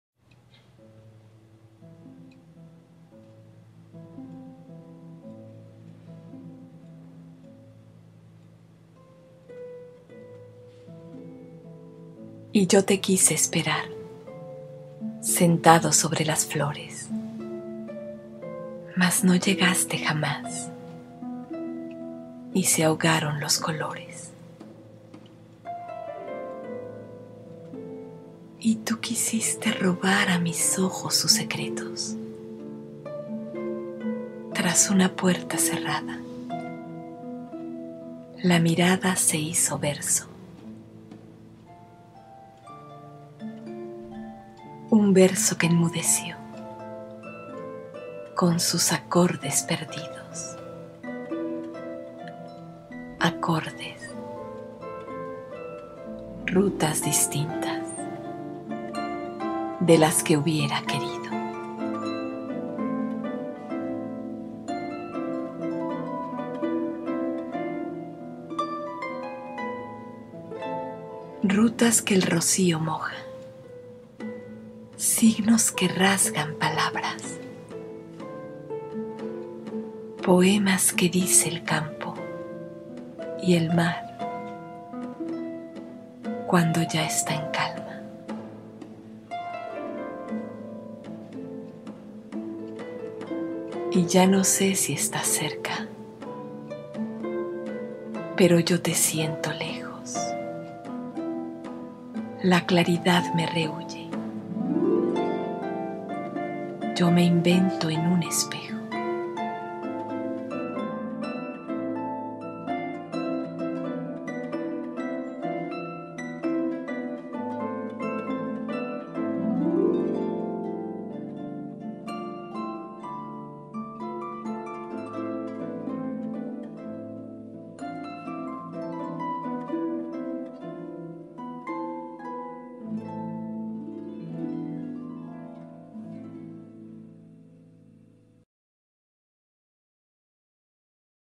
Canción 2 para María, de Antonio García Teijeiro
Y yo te quise esperar
sentado sobre las flores,
mas no llegaste jamás
y se ahogaron los colores.

Y tú quisiste robar
a mis ojos sus secretos.
Tras una puerta cerrada
la mirada se hizo verso.

Un verso  que enmudeció
con sus acordes perdidos.
Acordes, rutas distintas
de las que hubiera querido.

Rutas que el rocío moja.
Signos que rasgan palabras.
Poemas que dice el campo
y el mar, cuando ya está en calma.

Y ya no sé si estás cerca,
pero yo te siento lejos.
La claridad me rehúye.
Yo me invento en un espejo.

Canciones para María
Voz: María García Esperón
Imágenes: el manuscrito del poema cedido por Antonio García Teijeiro
MMX